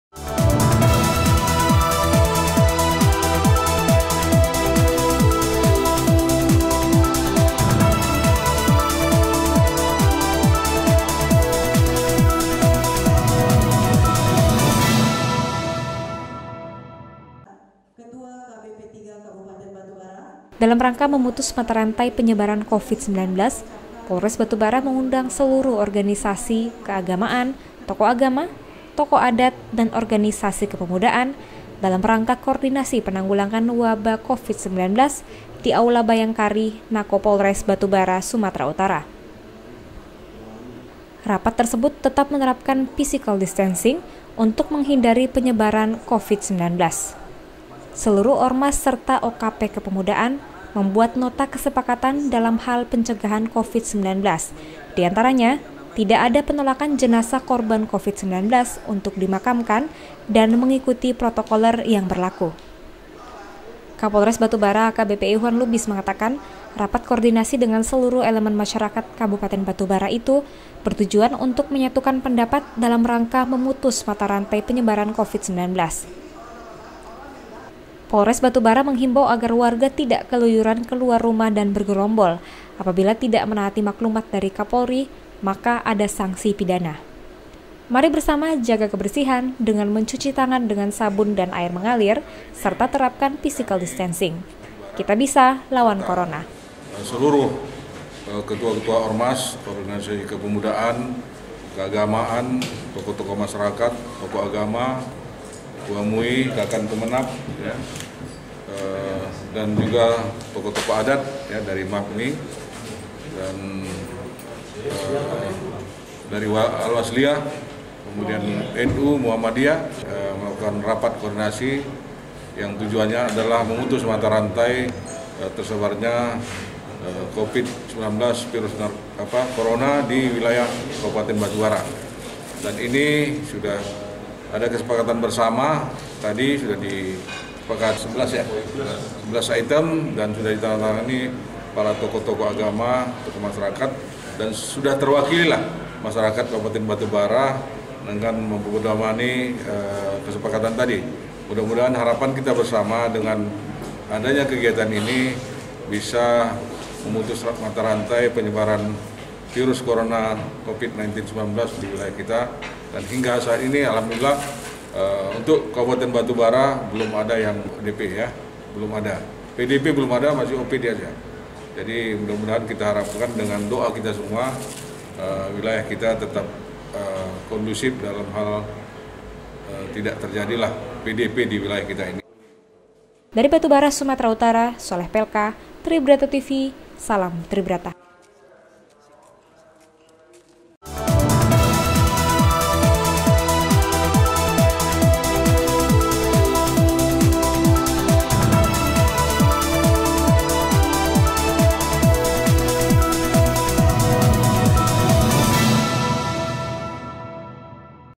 CEGAH COVID-19 KAPOLRES BATUBARA RAKOR DENGAN ORMAS & OKP
DALAM RANGKA MEMUTUS MATA RANTAI PENYEBARAN CPVID - 19 POLRES BATU BARA MENGUNDANG SELURUH ORGANISASI MASSA KEAGAMAAN TOKOH AGAMA TOKOH ADAT
DAN ORGANISASI KEPEMUDAAN DALAM RAPAT KOORDINASI PENANGGULANGAN WABAH VIRUS CORONA DI AULA BHAYANGKARI NAKOPOLRES BATU BARA SUMATERA UTARA